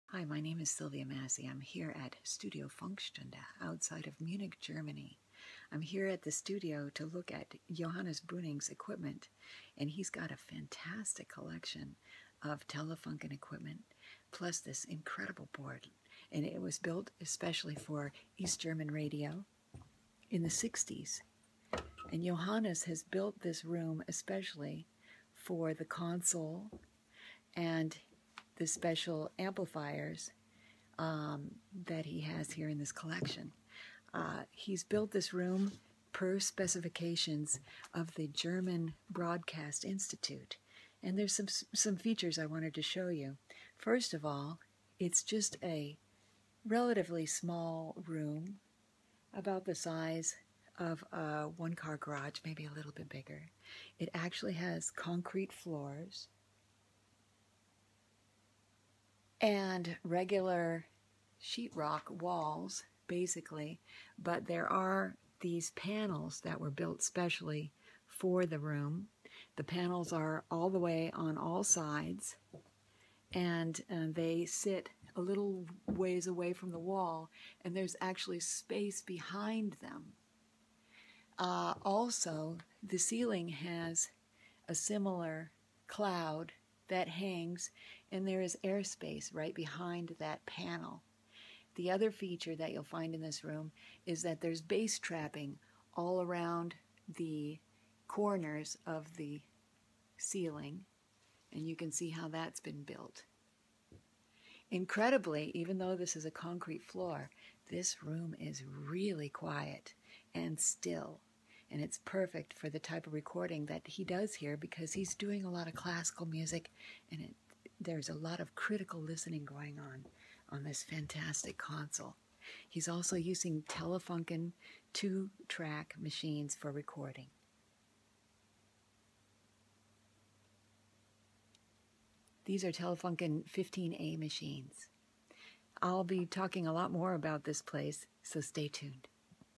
German Radio Control Room Design - with Sylvia Massy
While visiting a radio recording museum in Germany, Sylvia Massy describes how the control room of the recording studio was built.